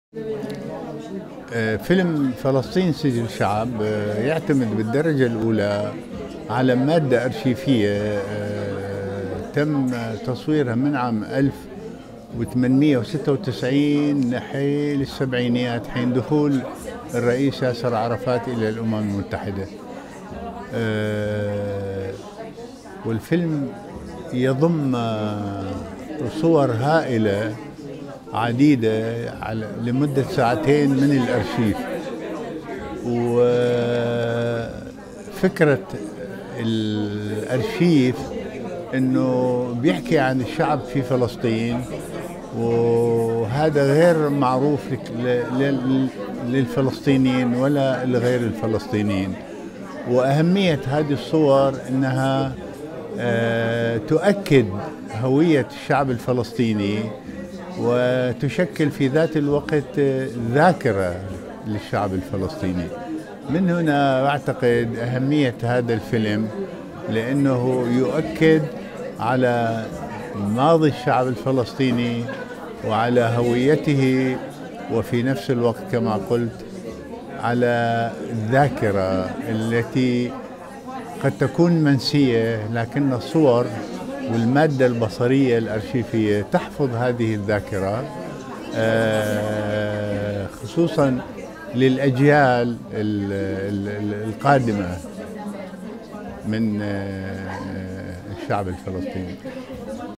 PALESTINE : FILMER C'EST EXISTER 2017 - Interview Kais al Zubaidi
Dans le cadre de la 6ème édition des Rencontres cinématographiques PALESTINE : FILMER C'EST EXISTER (PFC'E)
Interview du réalisateur irako-palestinien Kais al Zubaidi
Novembre 2017